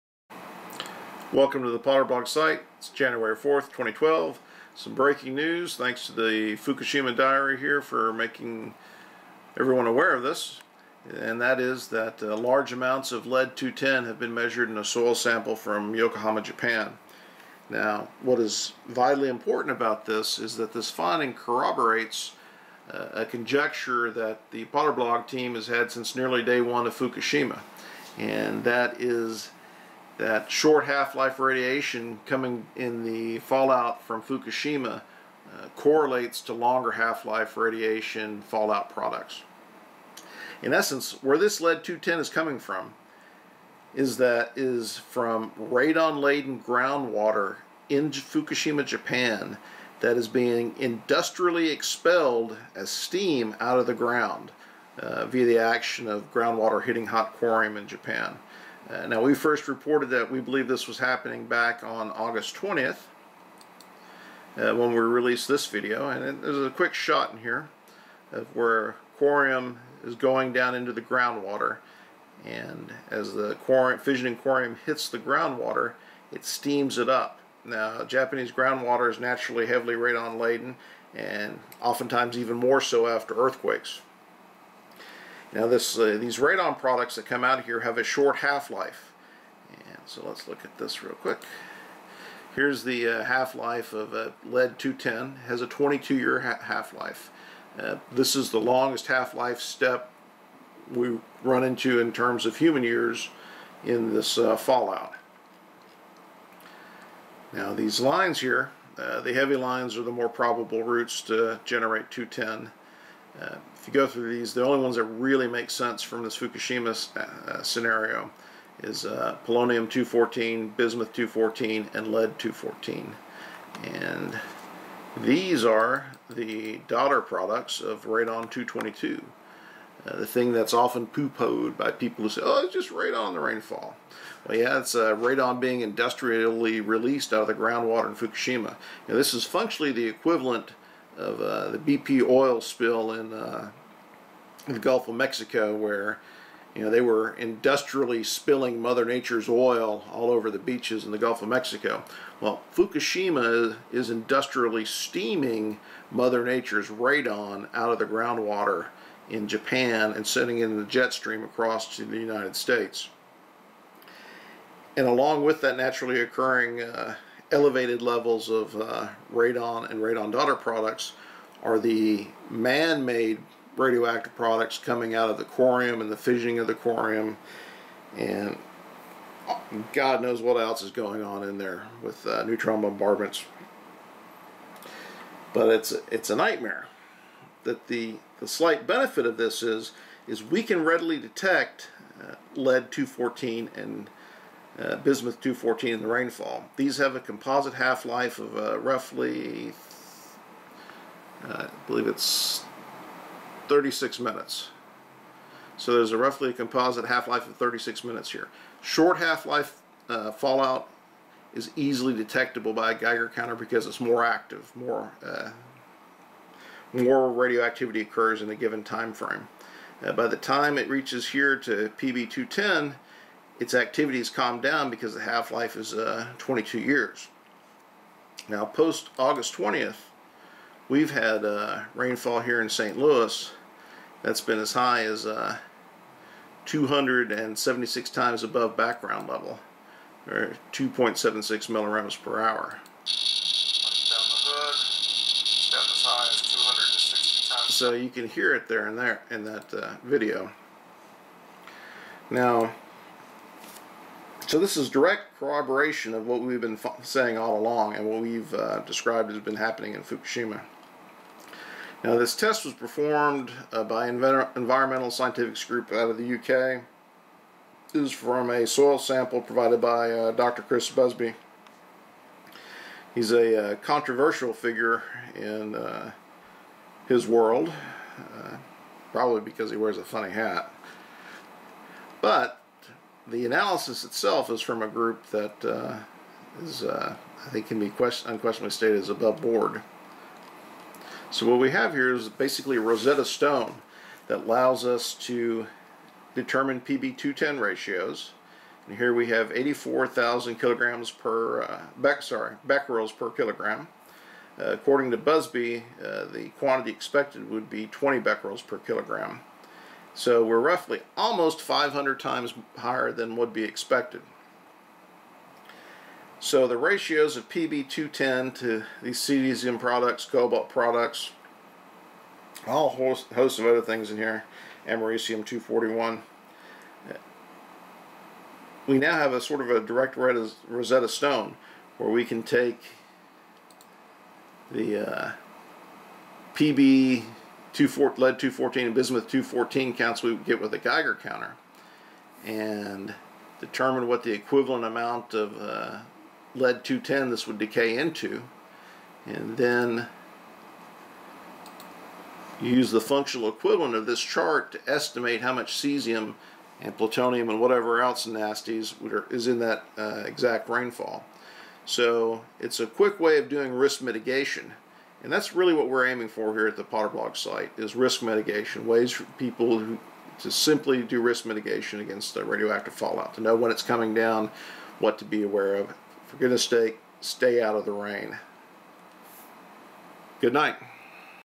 [Breaking] Busby Radioactive Lead Findings Corroborate POTRBLOG Fukushima Fallout Findings
The Fukushima Diary has reported the unusual findings of large amounts of radioactive lead 210 in soil samples from Yokohama Japan, approximately 500 times greater than would be expected.  These findings are a direct corroboration of the POTRBLOG team's analysis that Fukushima corium hit Radon laden groundwater and is releasing massive amounts of Radon in the resultant steam  along with all the harder to detect longer half life man-made Fukushima fallout.

Moreover, the detections of Cesium and other fallout in the soil sample build the foundation for a Rosetta stone which will allow one to infer and calculate suspected Fukushima radioactive contamination by taking Geiger Counter measurements of short half life Radon daughter fallout.